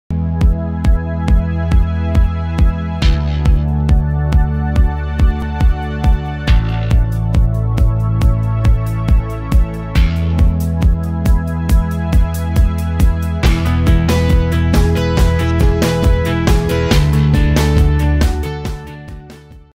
Bota Over The Knee Feminina Bebecê - 6010447712
Bota Over The Knee Feminina Bebecê

Bota Over The Knee Bebecê com cano super alto 3912-102, em material sintético. Oferece zíper na lateral. Cano com recorte na parte traseira, além de franja e tacha. Forro e palmilha são confortáveis. Já o solado é em borracha com salto robusto.

Medidas aproximadas
Altura do Salto: 5cm | Altura do Cano: 53cm | Circunferência: 40cm | Largura do Solado: 9cm | Peso: 1.154g